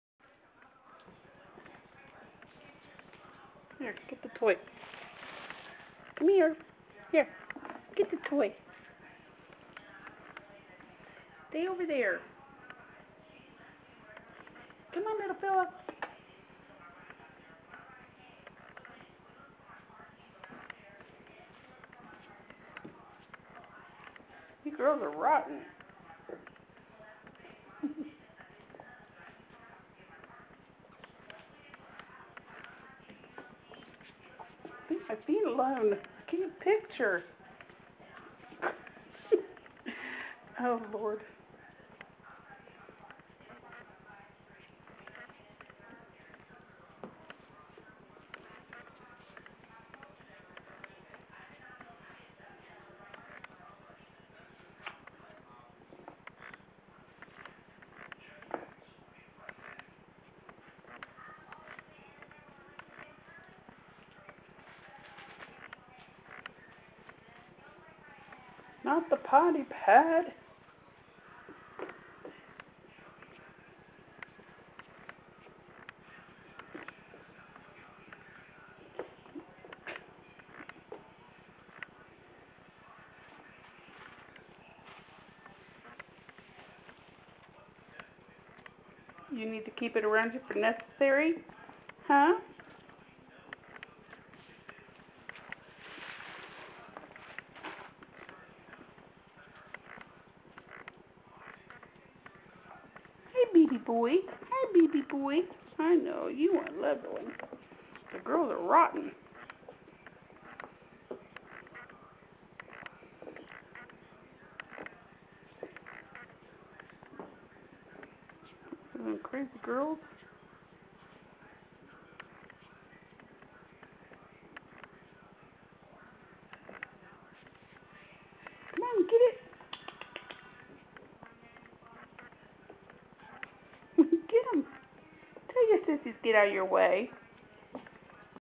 tashi pups playing 001
AKC reg. shih tzu puppies playing. These puppies will be Imperial size.